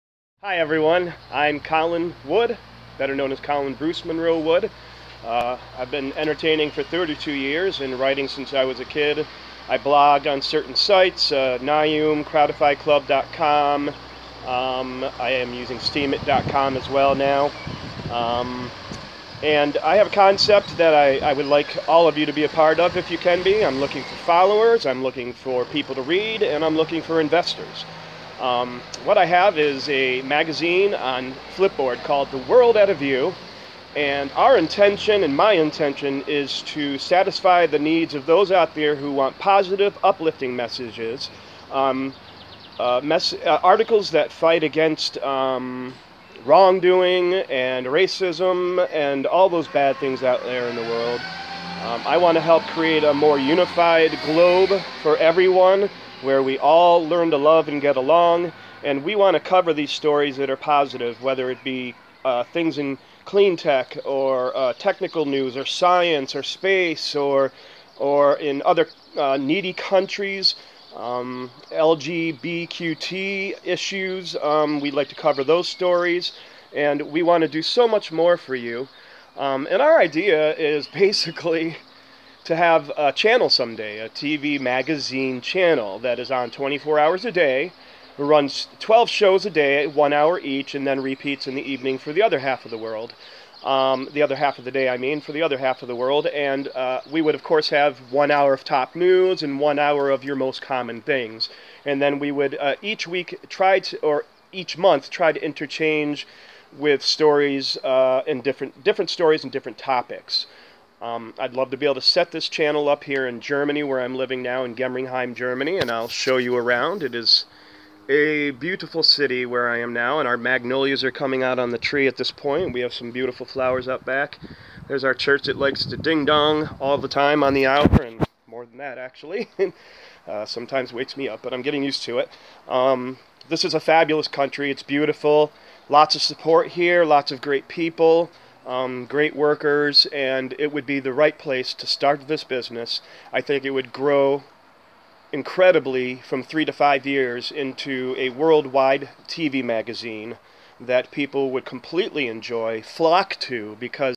Invest in The World At A View 24 Hour TV Magazine Channel
We would love for you to invest as little or as much as you can and want in a positive 24/7 Magazine Channel in Germany, that will spread across the Globe. We are drawing up our busines plan for anyone interested, and we can contacted at , if you would like more information. Please be specific if you can. If you just want to be a follower, reader of our current Magzine - The World At A View on Flipboard, or a viewer of our future show, please follow us here and on our Twitter, and Facebook accounts. We plan to show uplifting, positive news, shows, topics, and more. We would like to help make the World a more Unified Globe of individuals! Thank you for your interest and for watching this video.